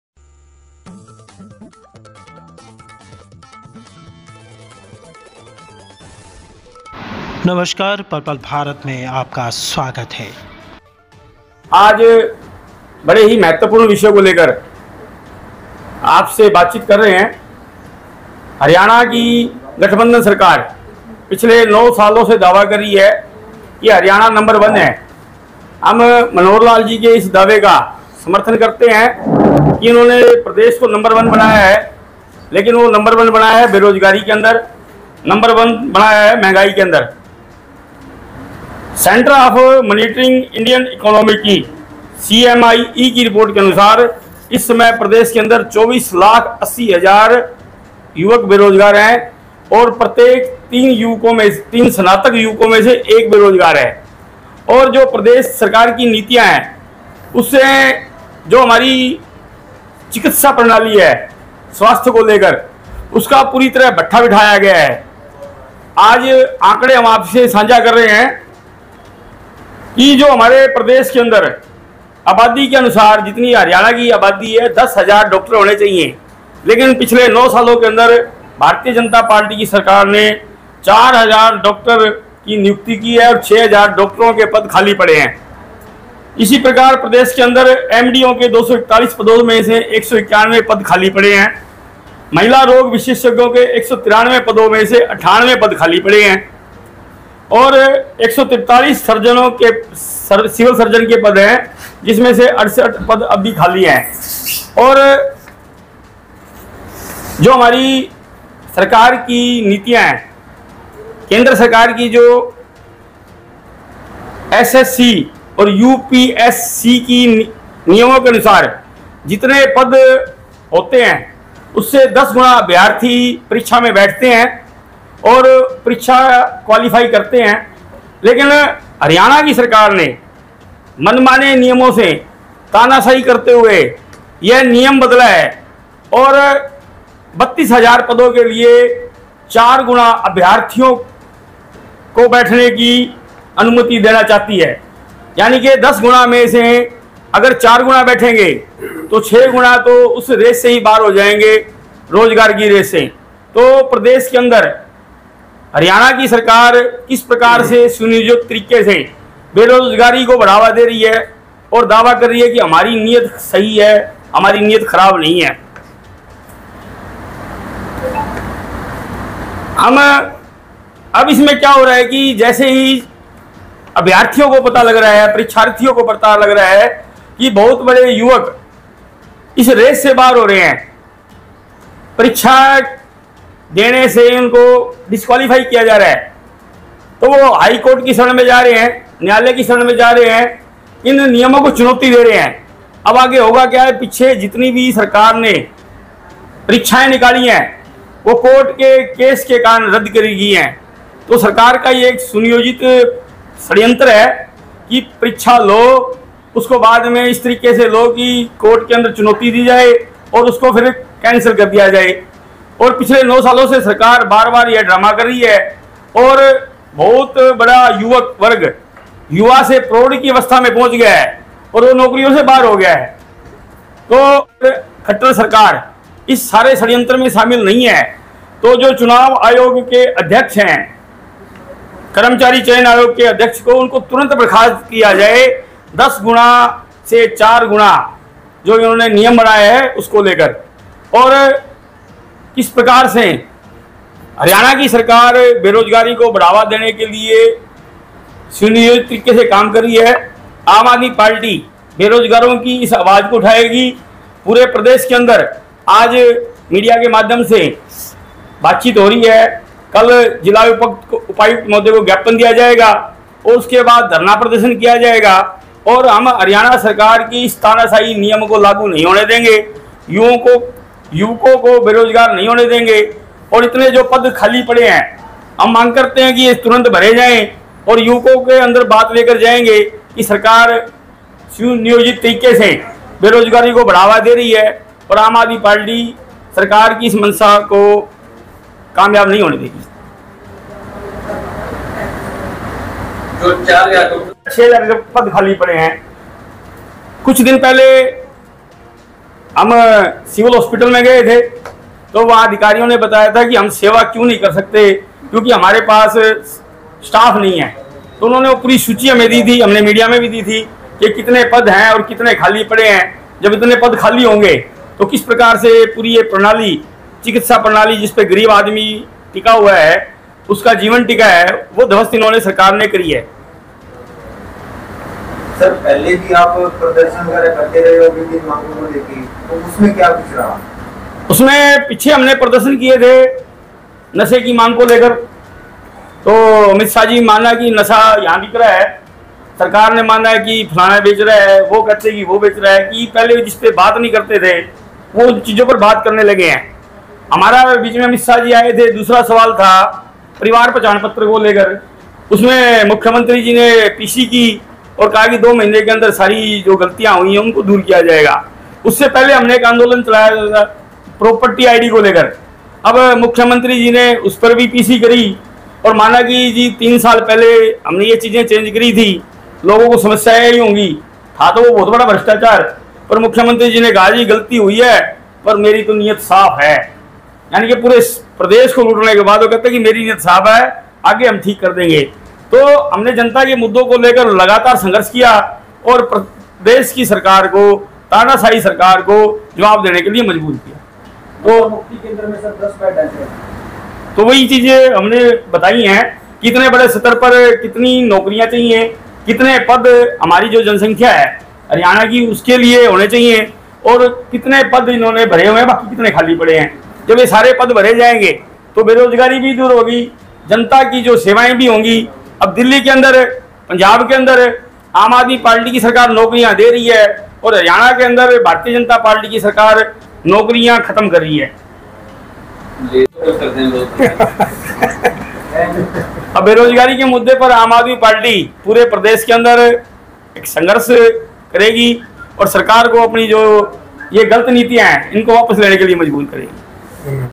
केंद्रीय परीक्षाओं में उम्मीदवारों कि सीटों को किया सीमित- सरकार की मनमानी
@palpalsirsa सिरसा aap amadmiparty केंद्रीय परीक्षाओं में उम्मीदवारों कि सीटों को किया सीमित- सरकार की मनमानी